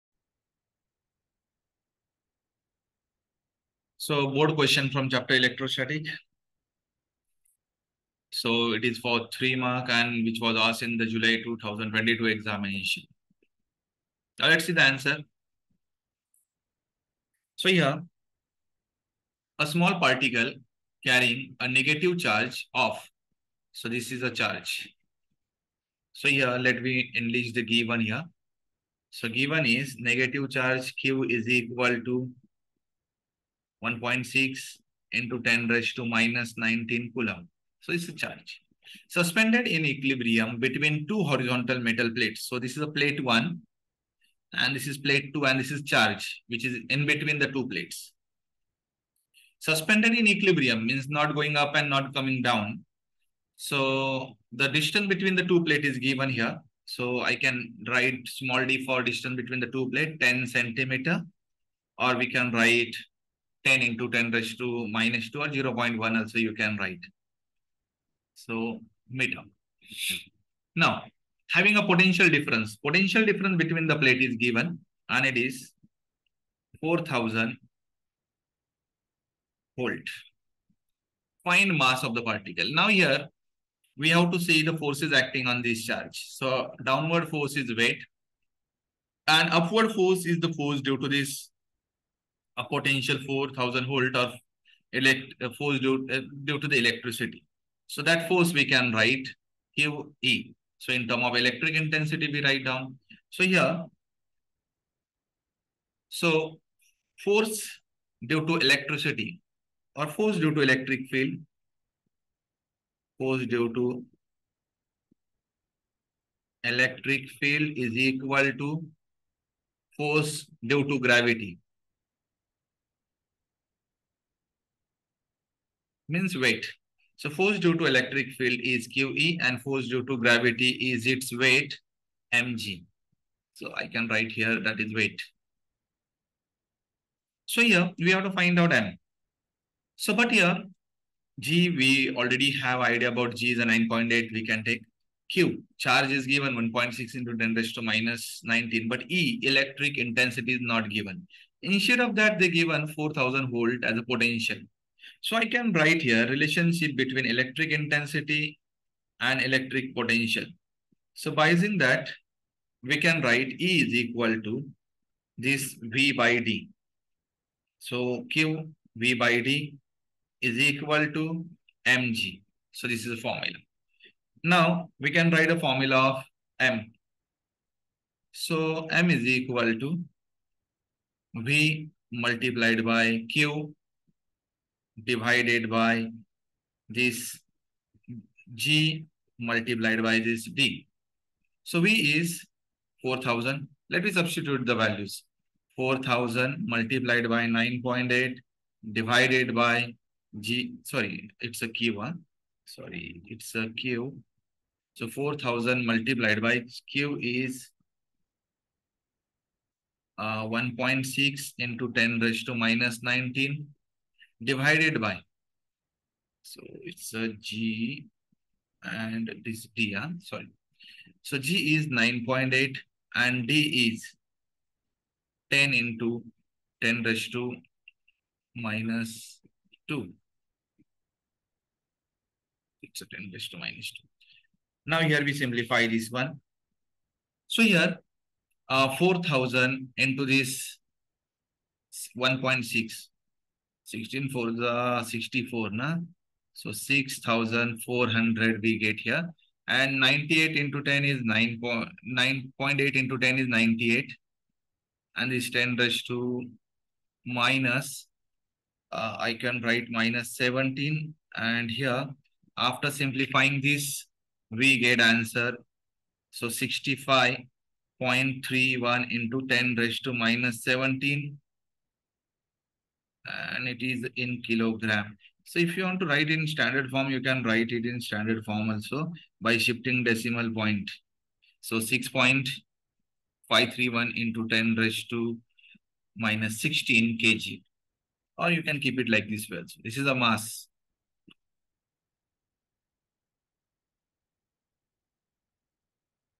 A small particle carrying a negative charge of 1 6 x 10 19 C is suspended in equilibrium between two
A small particle carrying a negative charge of 1.6 x 10-19 C is suspended in equilibrium between two horizontal metal plates 10 cm apart, having a potential difference of 4000 volts across them. Find the mass of the particle.      (July 2022)

hsc board
Maharashtra state board
Physics
Physics by mahendra rokade
Physics by science hub
previous year Question
mahendra rokade
science hub